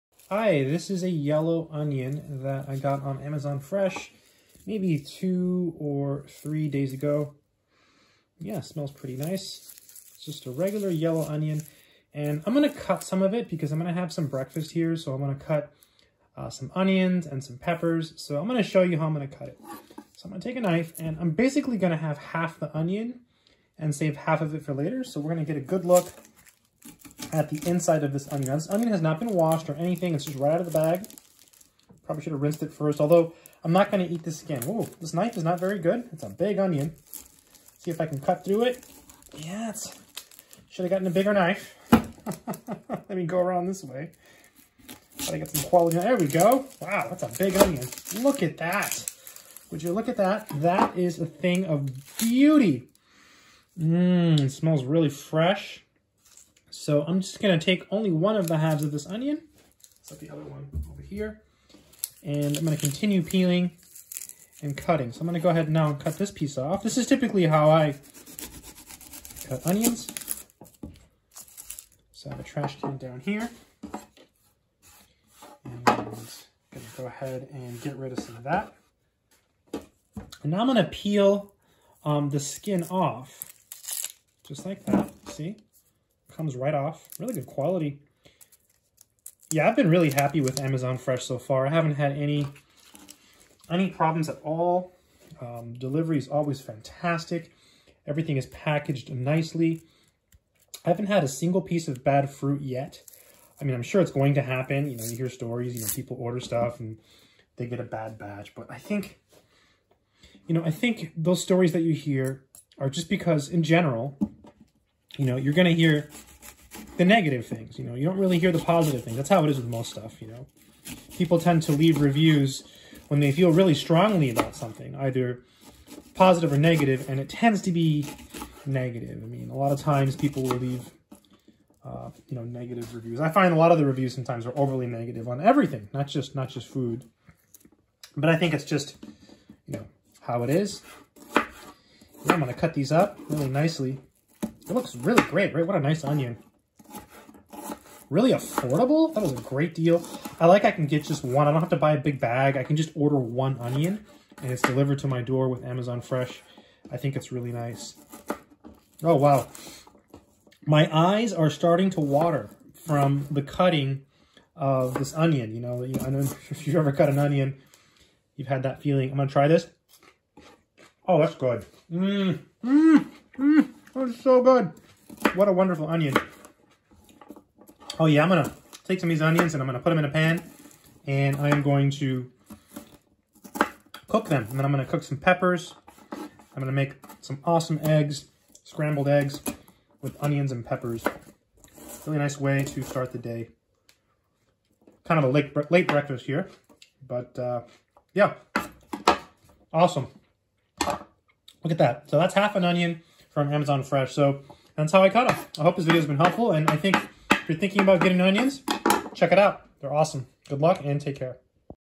Yellow Onion
In this video I talk about a very nice yellow onion I got.

This is the yellow onion on amazon